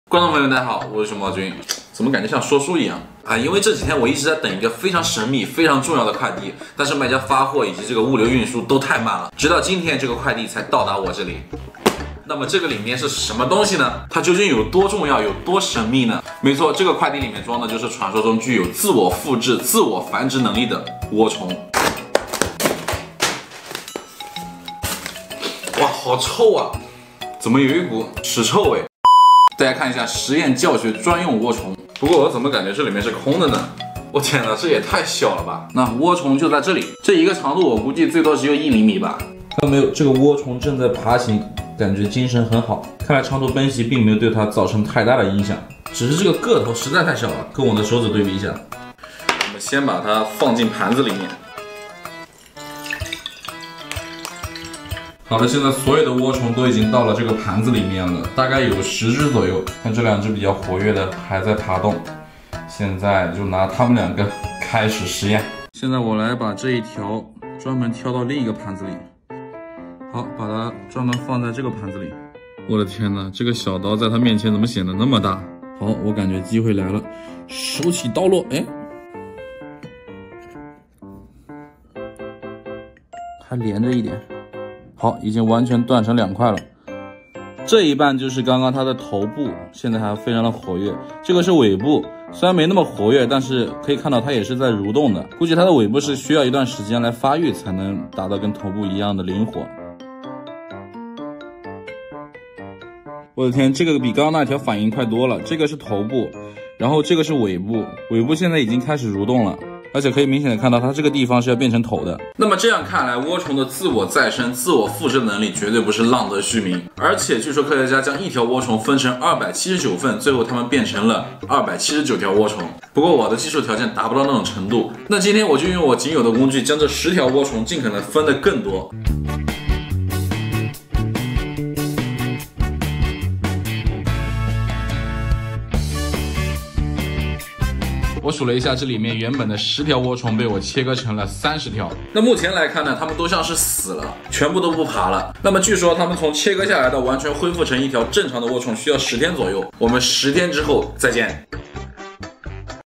开箱一个具有超强再生能力的神秘生物，把它切成279块都不死？【熊猫君来啦】
熊猫君买了十只传闻中的，具有自我复制、自我繁殖超强的涡虫！科学家曾经将一只涡虫切成了279段，而这279段涡虫都长成了一只完整的涡虫！就让我们一起来一下吧！
涡虫 再生能力 自我繁殖 自我复制 本视频完全符合YouTube社区准则，没有任何违规行为。